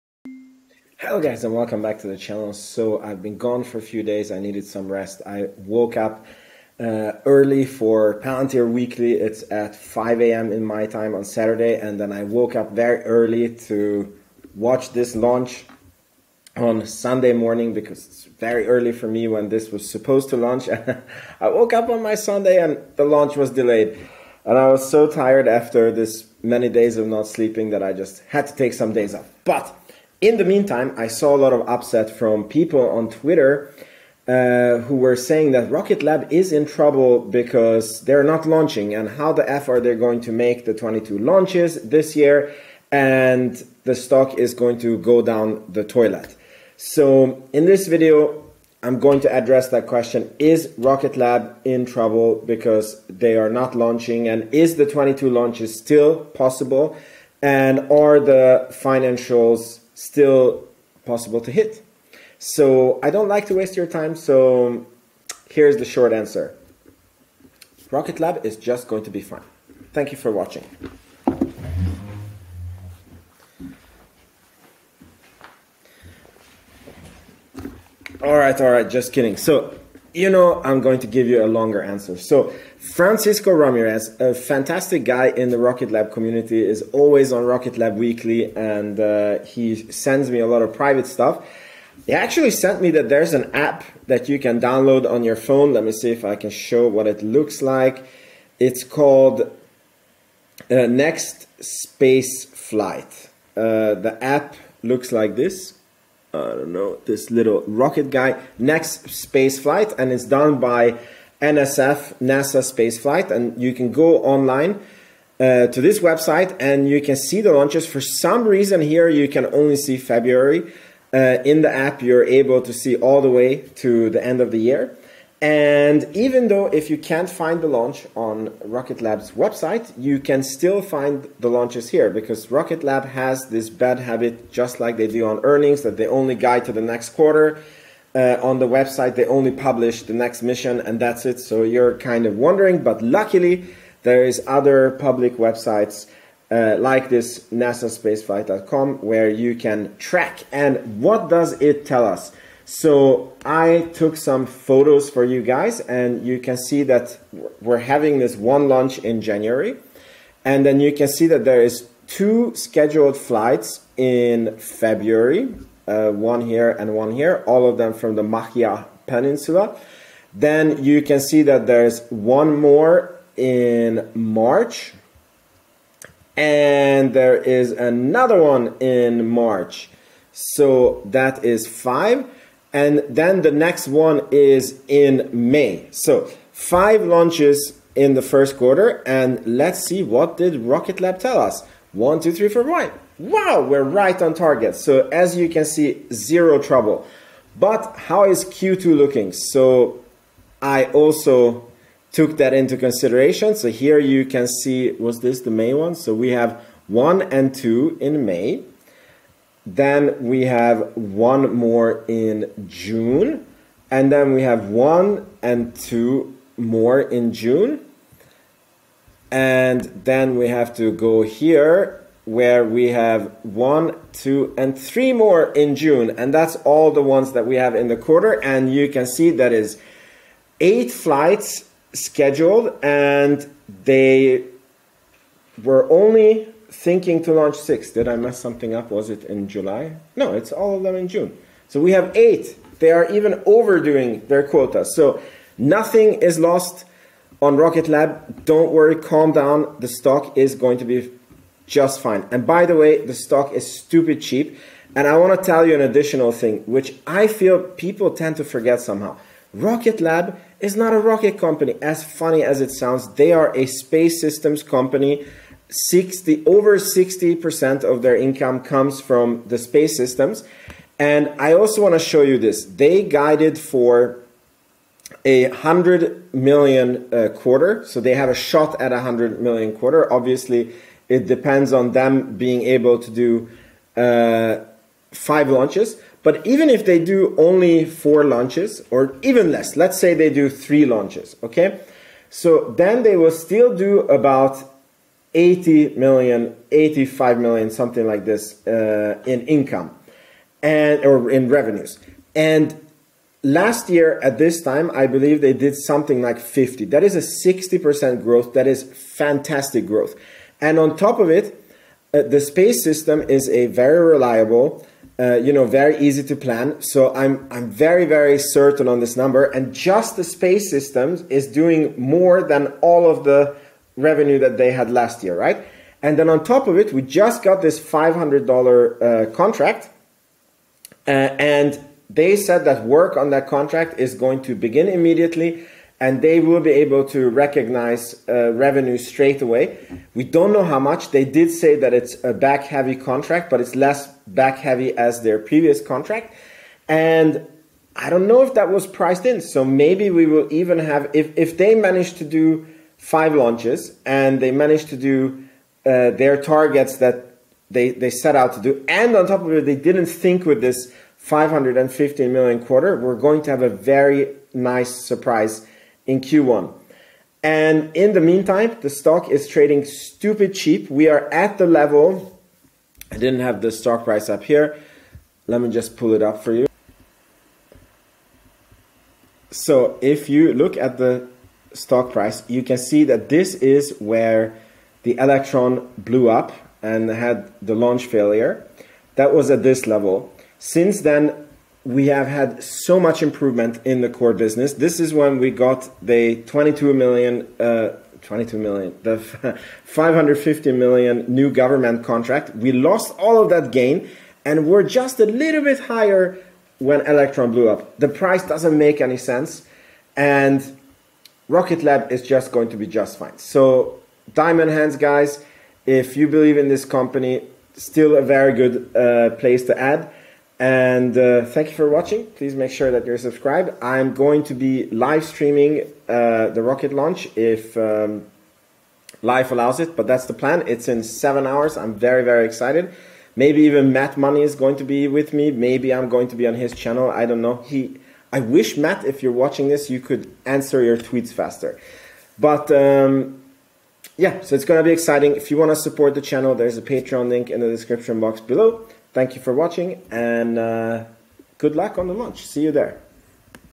Is Rocket Lab in trouble because of not being able to launch?
Follow me on X: @vinceisbullish

In this video I am going to talk about if Rocket Lab is in trouble with the Rocket Launches and if their target is still real to reach 22 launches.

The stock is crazy cheap and they are able to easily still meet their target.